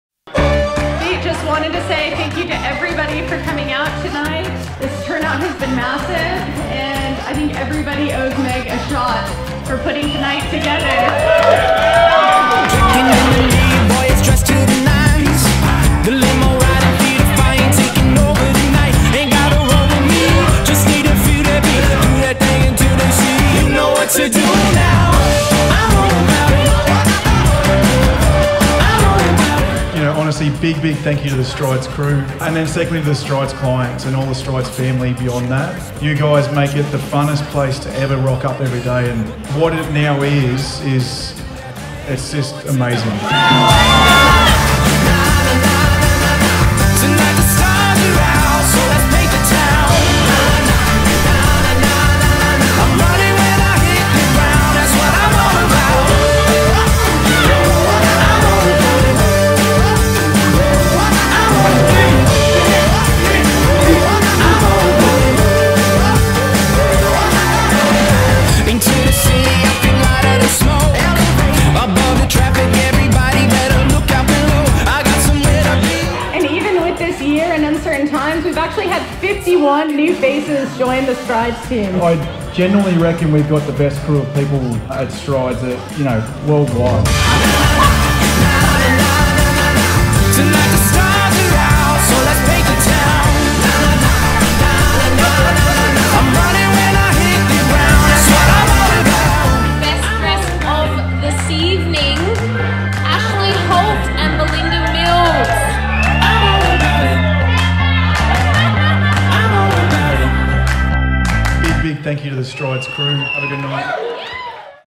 Making Strides 8th Birthday | Highlight Video
We celebrated 8 years of EPIC Making Strides this year, Oktoberfest style!!!